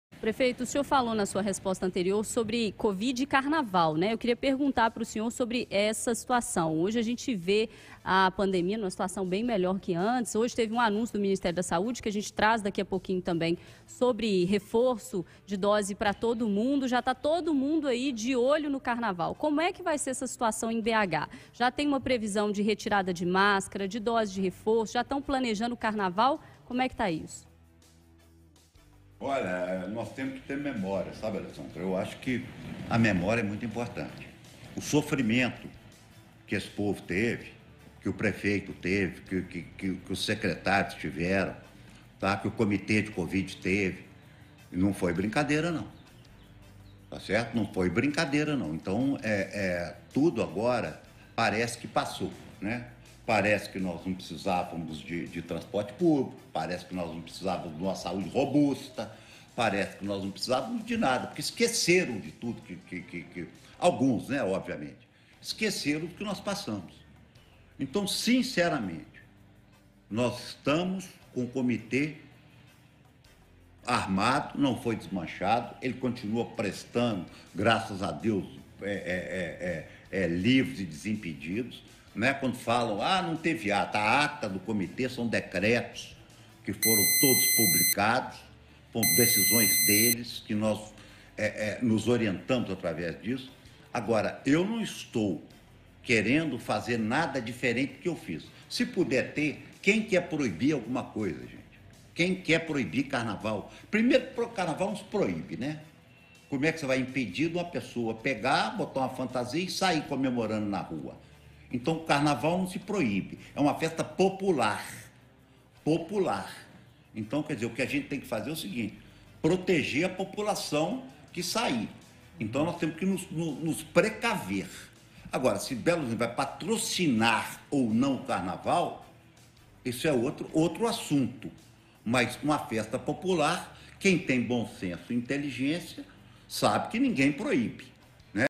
KALIL DIZ QUE PBH NÃO VAI PROIBIR O CARNAVAL EM 2022: 'TEMOS QUE PROTEGER A POPULAÇÃO QUE FOR SAIR'
Prefeito de BH disse, em entrevista exclusiva a Itatiaia, que comissão da covid ainda estuda se Prefeitura irá patrocinar festividade. Confira!

📲 Twitter.com/radioitatiaia
📸 Instagram.com/radioitatiaia
💻 Facebook.com/radioitatiaia